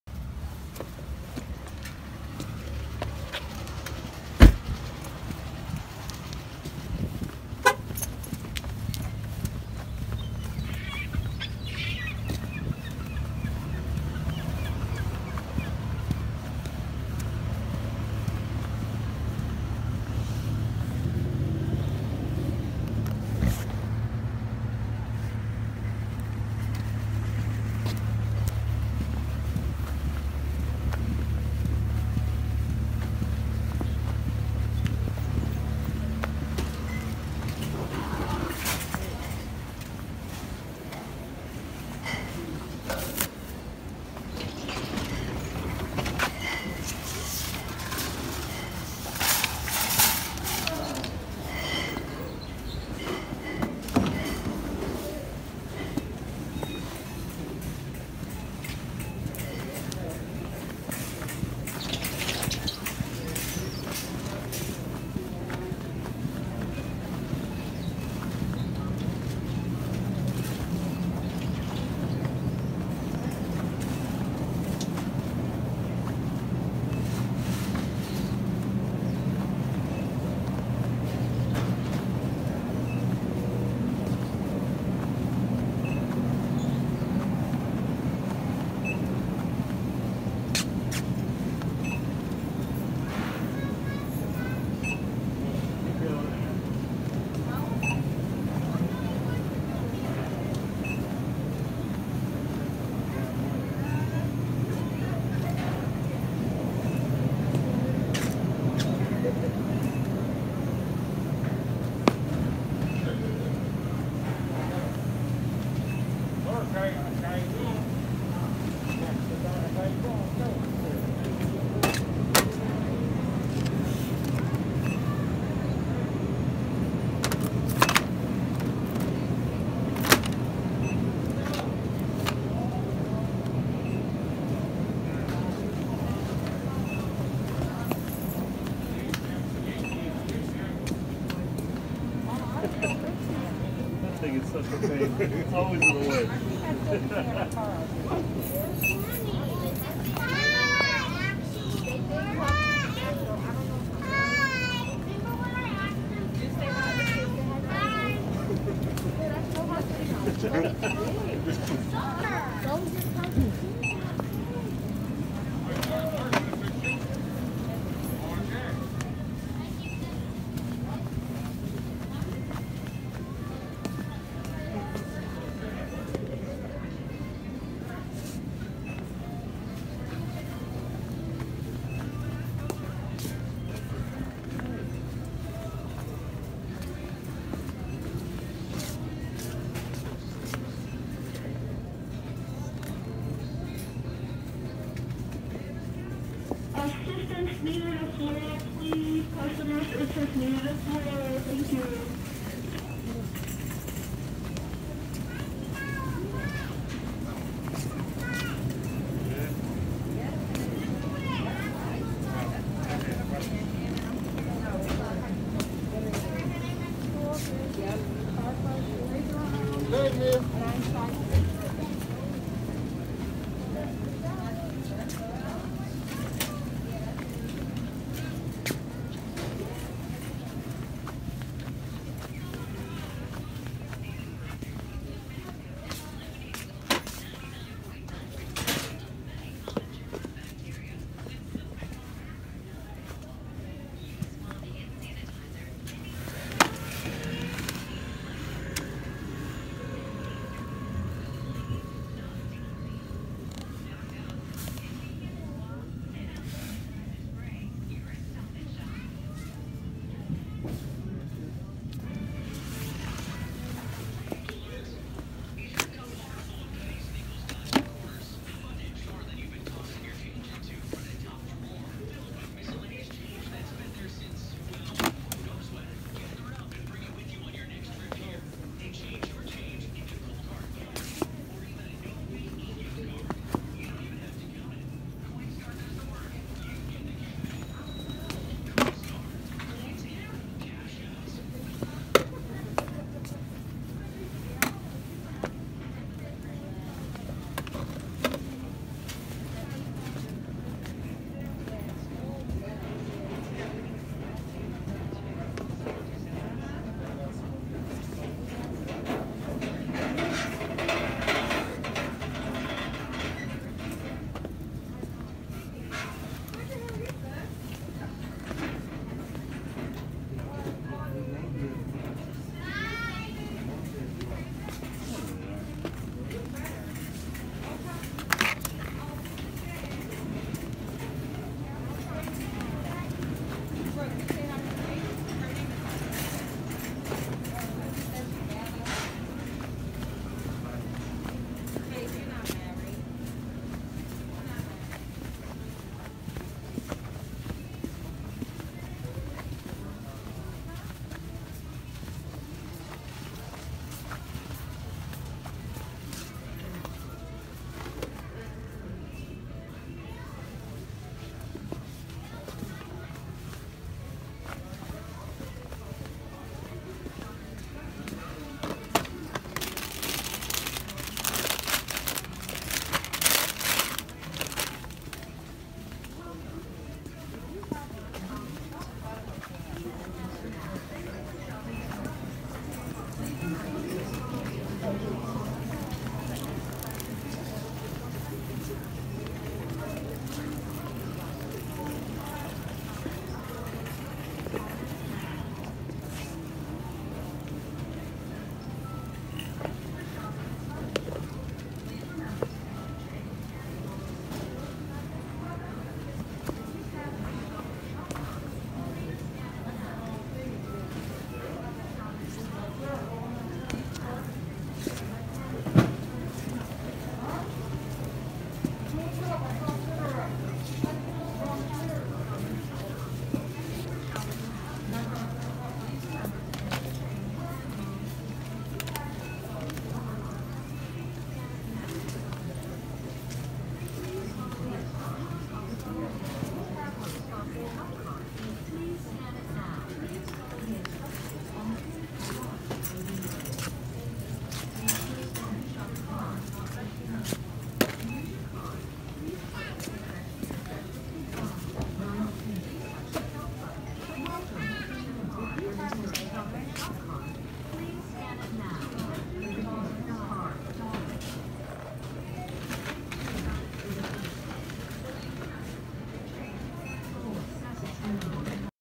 Grocery Shopping Wearing My Hot, Tongueless and Laceless Cole Haan Wingtip Dress Shoes
Decided to go grocery shopping wearing these nice and loose. Even though they are size 11, they were a little tight. Even with the laces and tongues yanked out!